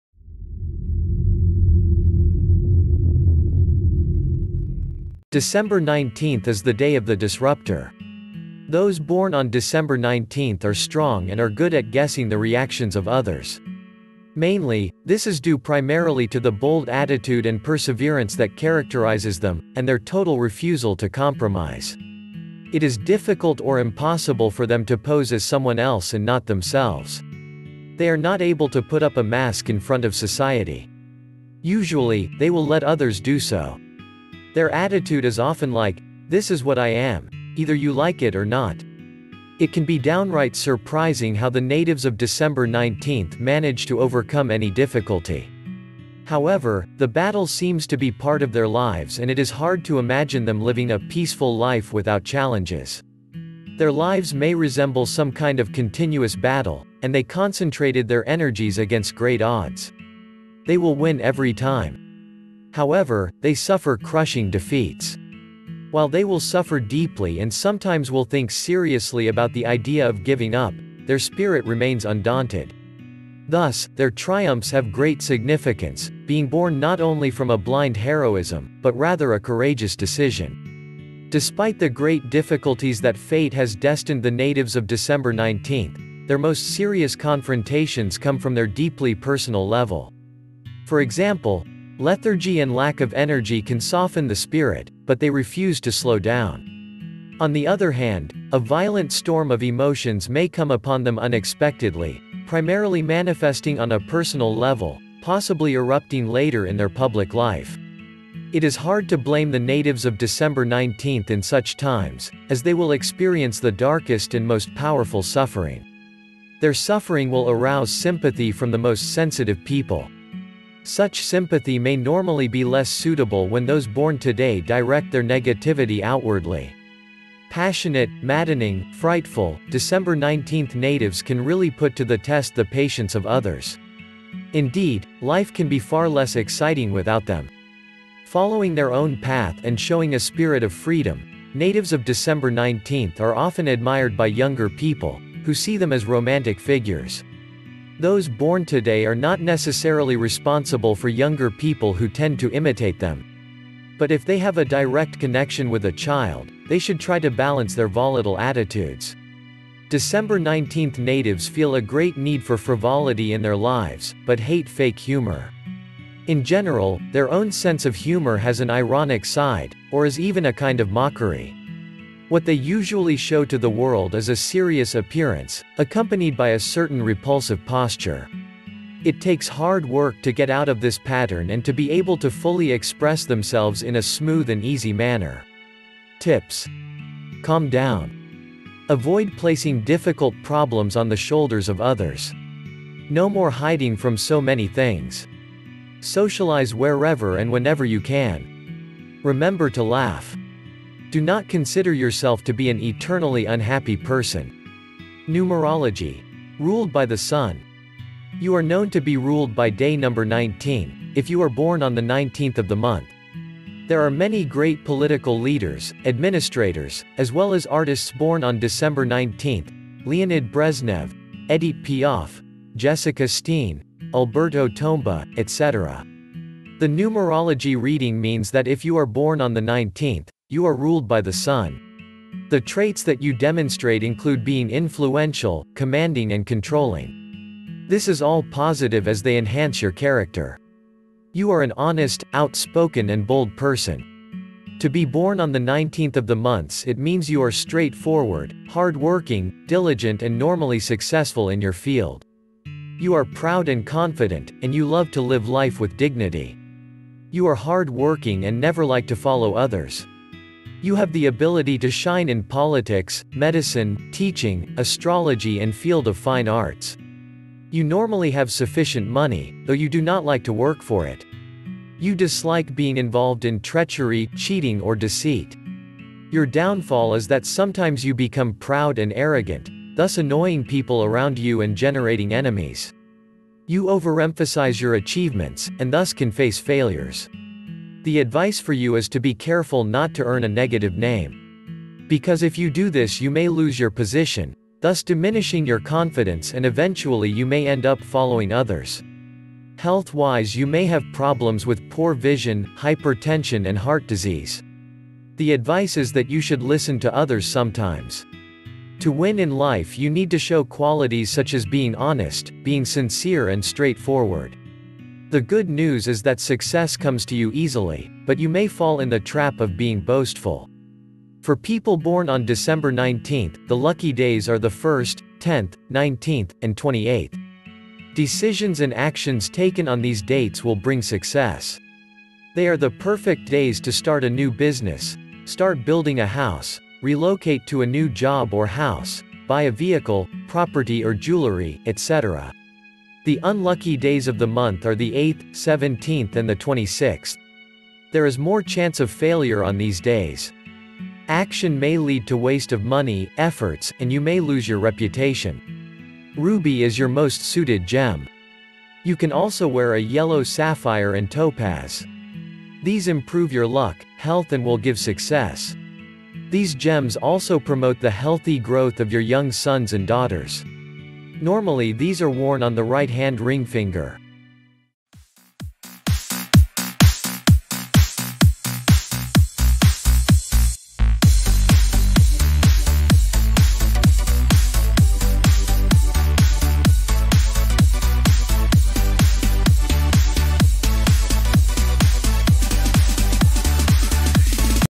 Born on the 19th of December | This is the SECRET OF YOU | Happy Birthday
If you are born on the 19th Of December this video is specially made for you.
This video will show why your birth date is important and how is your personality.